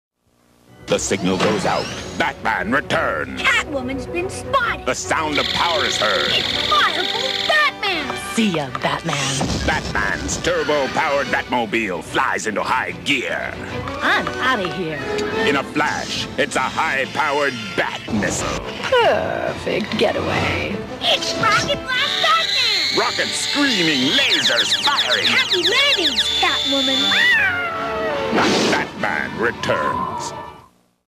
Batman Returns Toy Commercial 1 Catwoman Batmissile Firebolt Rocket Blast Kenner
Commercial for Kenner's Batman Returns toy line. This commercial focuses on Catwoman, Firebolt Batman, Rocket Blast Batman, and the Batmissile Batmobile. For more on Kenner's Batman Dark Knight Collection and Batman Returns toy lines, head